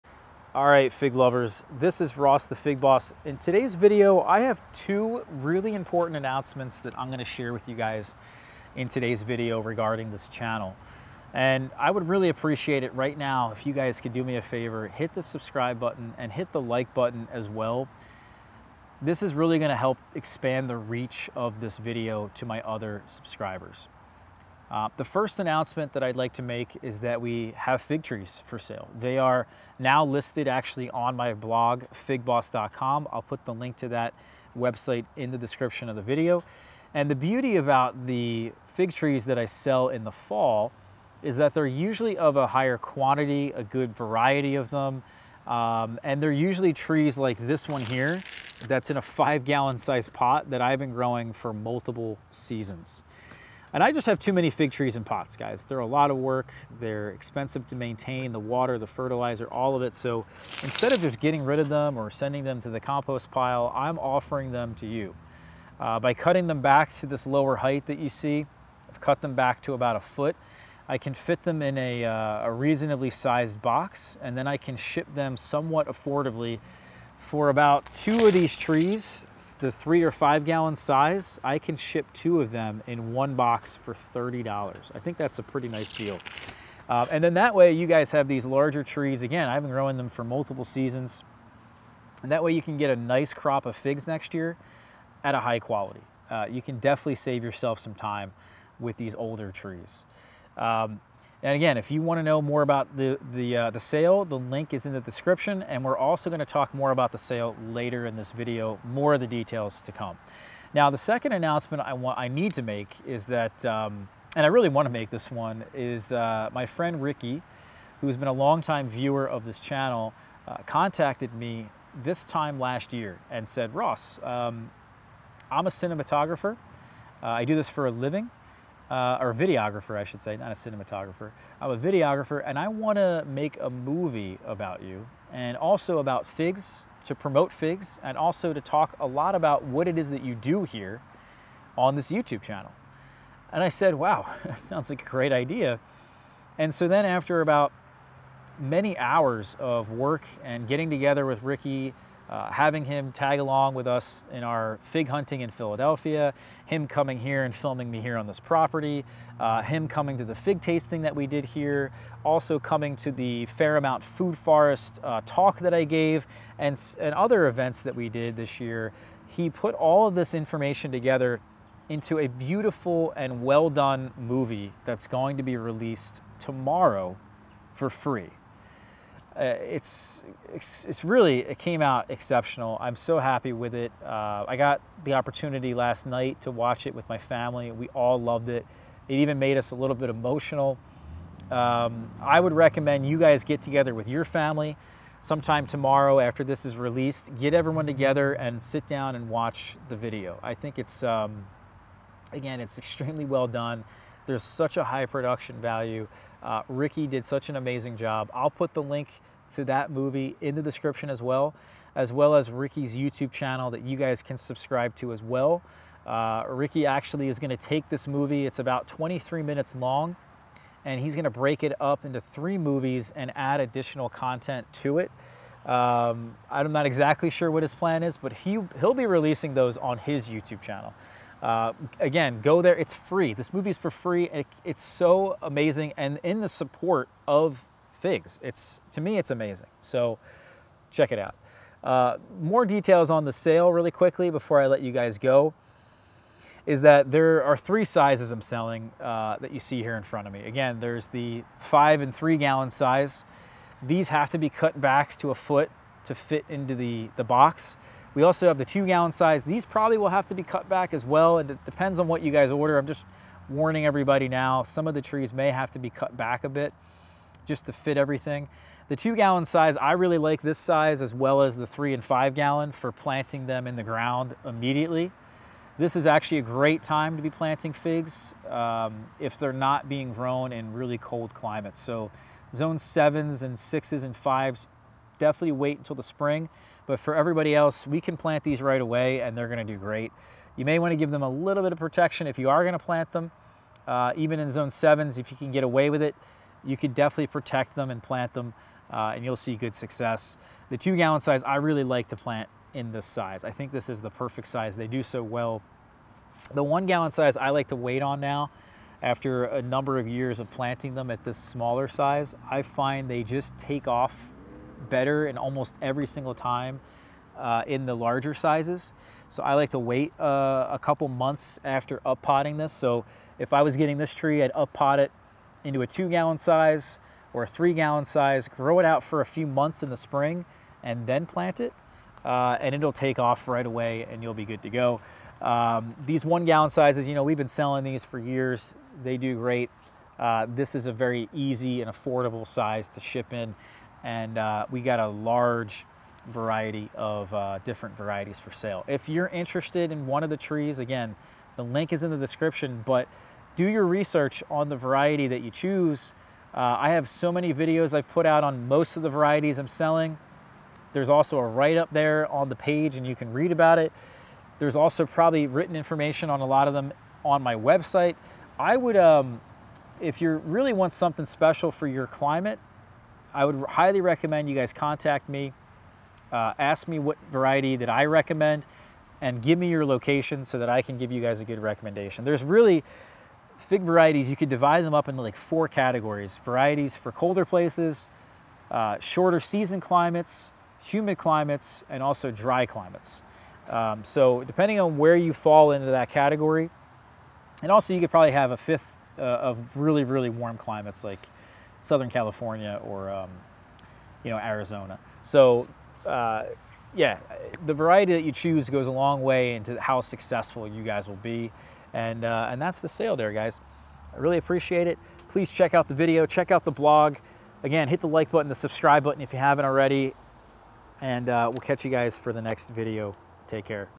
ANNOUNCEMENT: Fig Trees for SALE & Upcoming FILM Premiere - "The Fig Boss of Philadelphia"
DON'T MISS the film by Ricky Oliveras premiere, airing on Sunday, November 5th at 7 PM EST
To read about my 2023 Fig-filled Season, check out everything I've been up

"The Fig Boss of Philadelphia"

“Ross Raddi set off on a fig-growing journey, inspired by his grandfather. Now, he is known as the fig boss. Ross is on a mission to share his love of figs with the world while discovering and preserving unique fig varieties in Philadelphia.”

Take the time to sit down with your family for the premiere! You won’t want to miss this emotional and relatable film about figs.

He'll be publishing an additional 3 more videos on his YouTube channel!